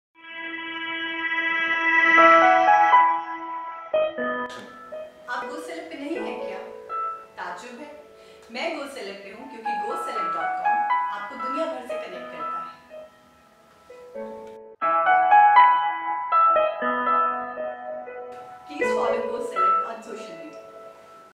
Sonam Lamba - GoCeleb
With her girl-next-door looks, Sonam has become a popular face on the TV scene. She began her career as Ritu Pandey on the popular serial Desh Ki Beti Nandini. She went on to play roles on famous shows like Balika Vadhu as Guli and Saath Nibhaana Saathiya as Vidya Modi among others.